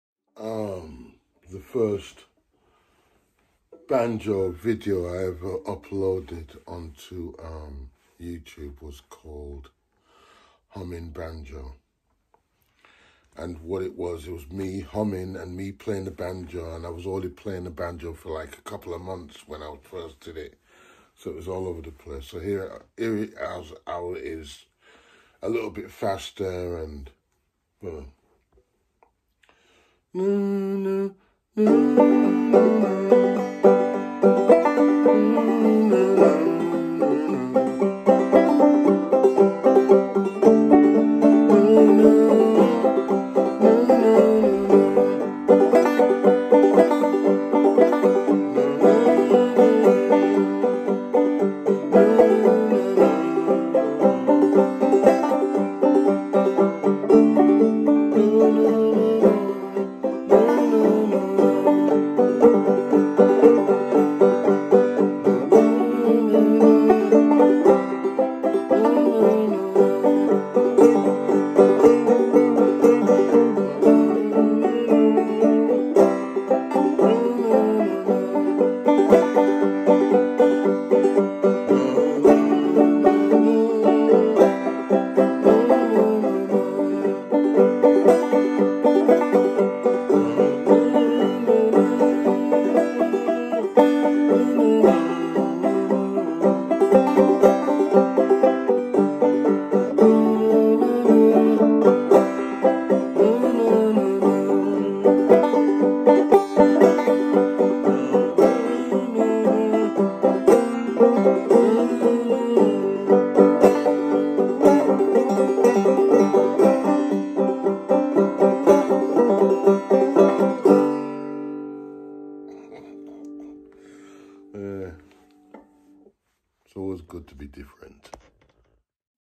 Humming True Banjo / clawhammer banjo
In fact this is the first Banjo video song I uploaded. It is the traditional tuning of G. Yeah my first ever ,🪕🪕🪕